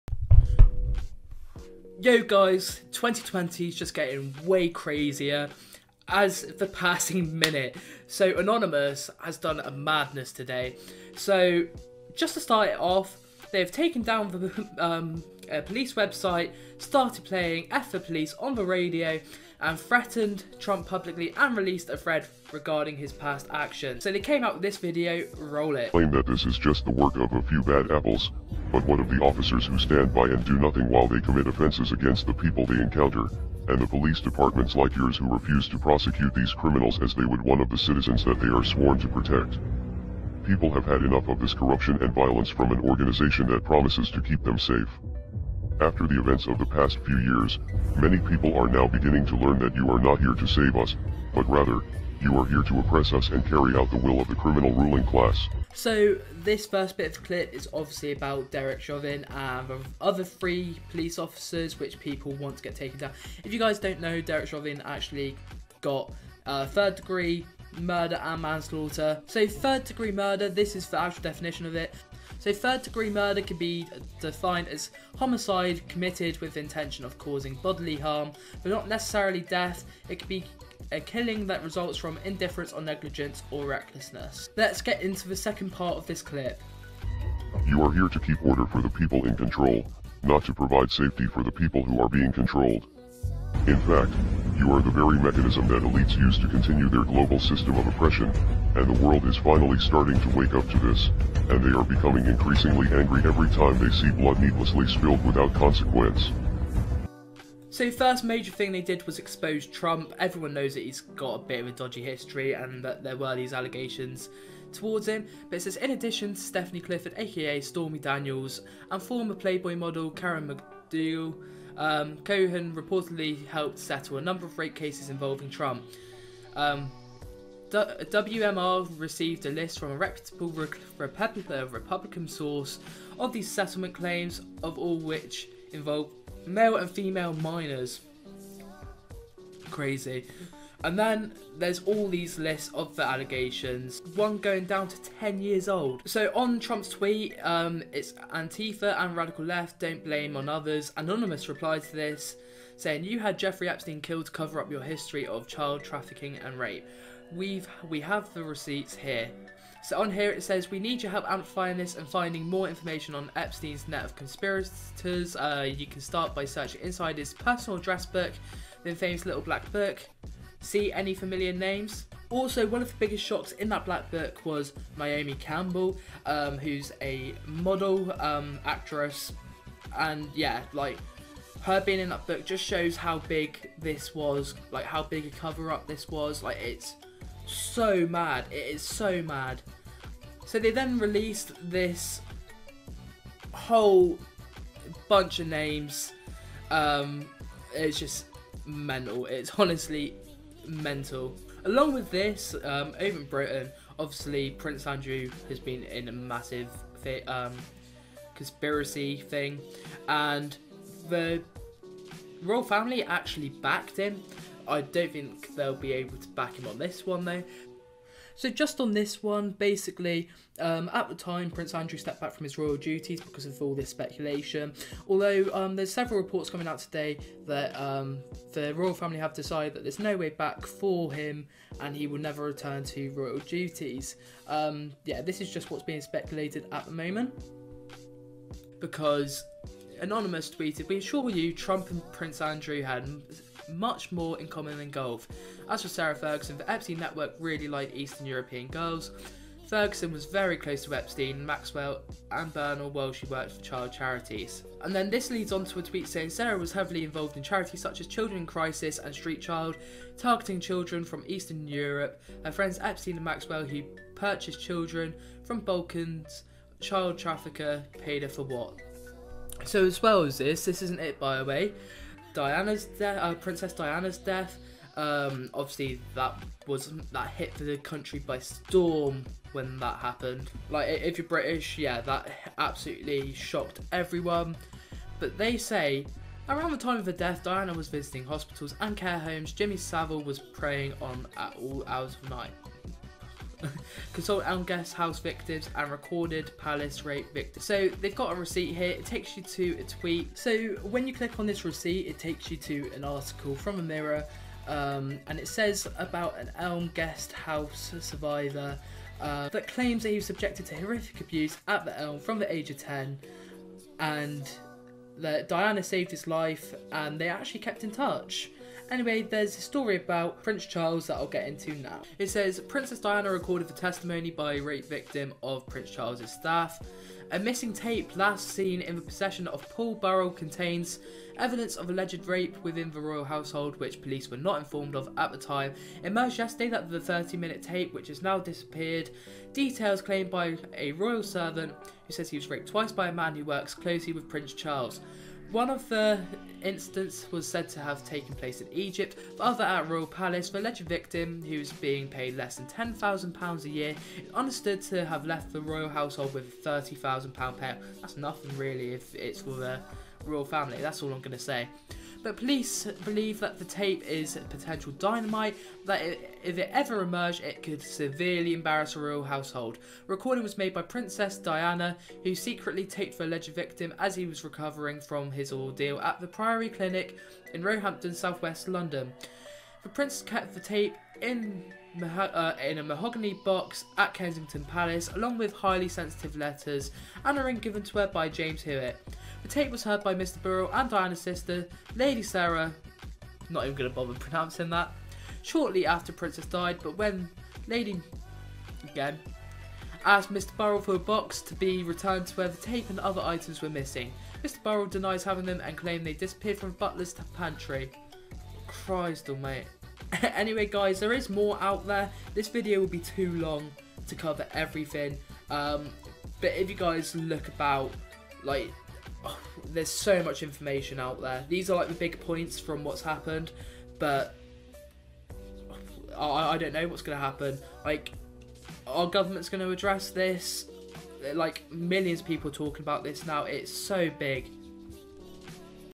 ANONYMOUS LEAKS HIDDEN DOCUMENTS (Hidden Truths Released?)
They released documents about Donald Trump, The Royal Family and much more...